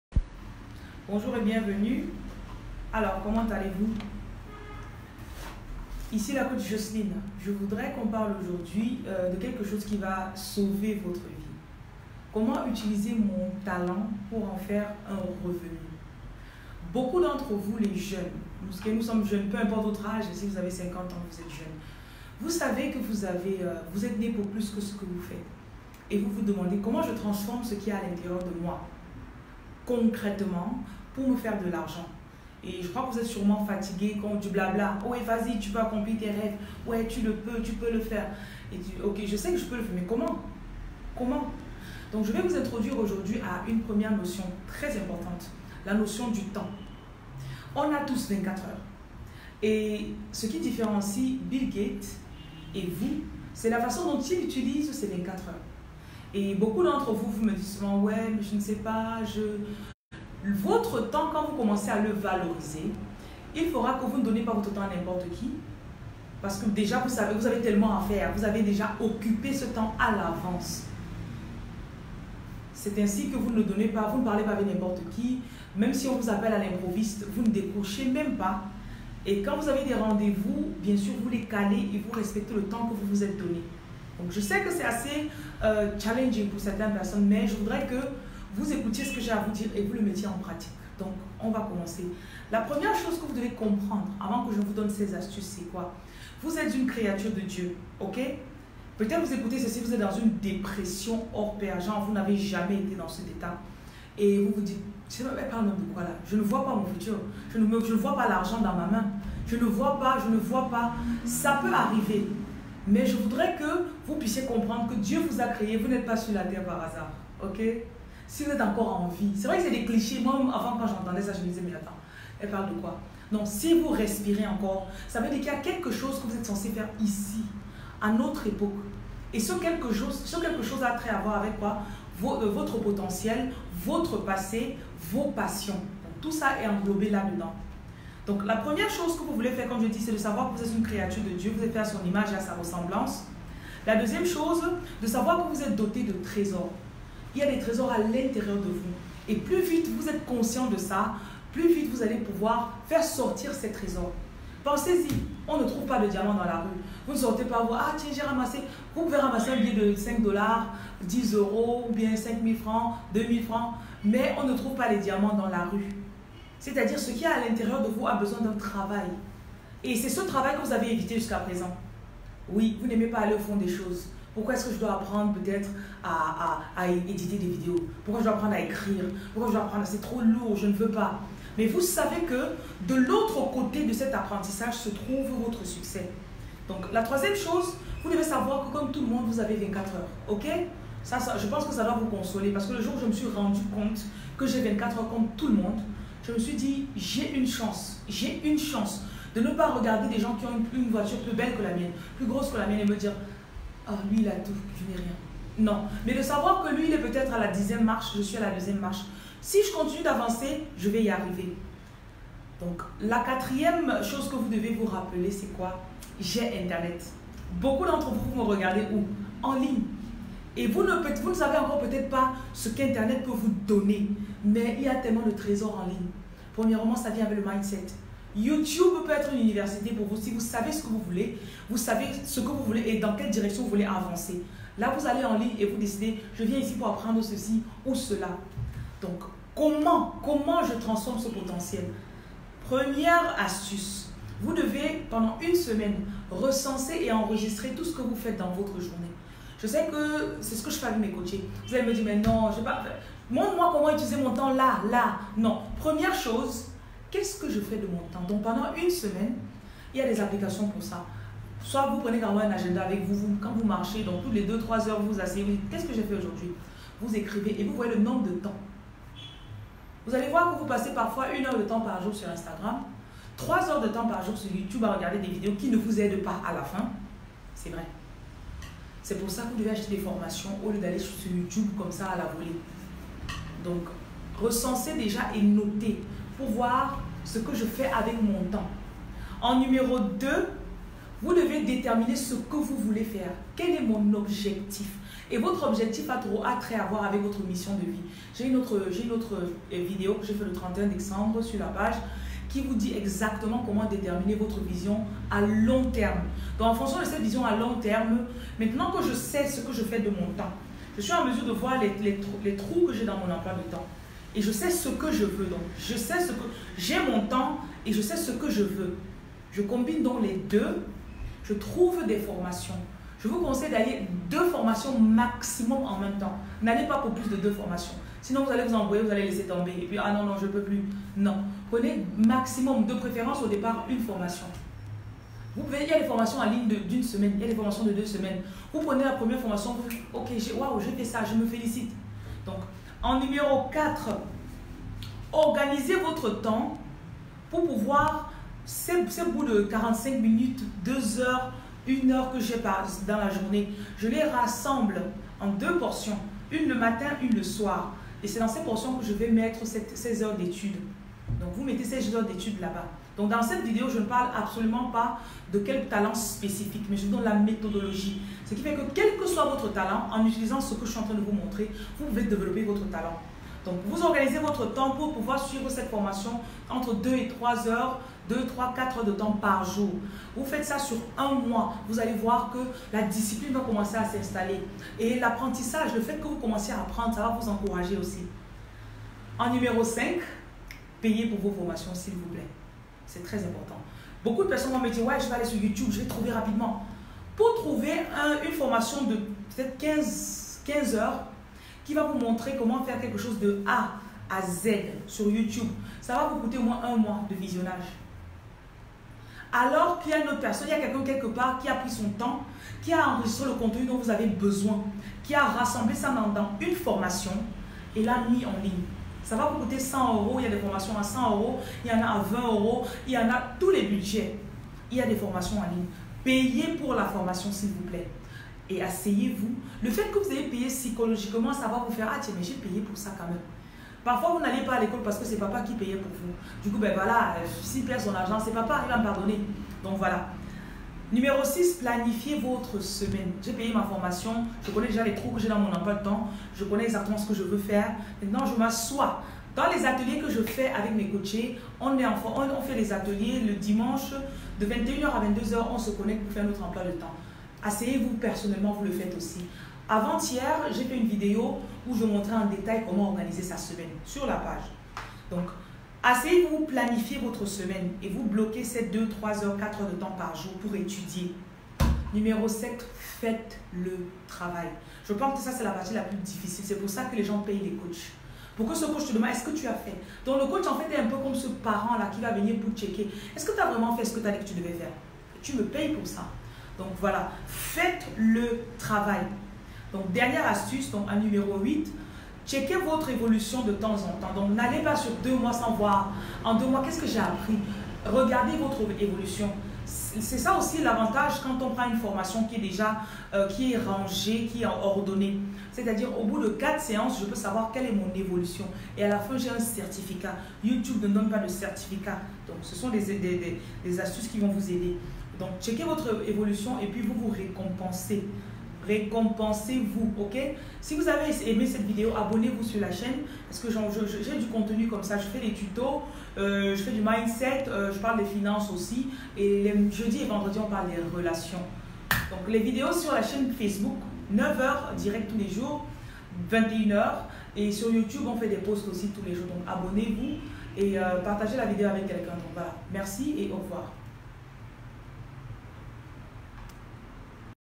Comment sortir de la pauvreté avec son talent?
De nos jours nous souffront à cause du manque d'exercice de notre talent or on peut mettre notre potentiel au service des personnes pour en retour recevoir l'argent de ceux ci . Ce que nous portons à l'interieur de nous est notre fortune sachons l'utiliser pour nous faire une place dans la société.

Pour prendre un rendez-vous : https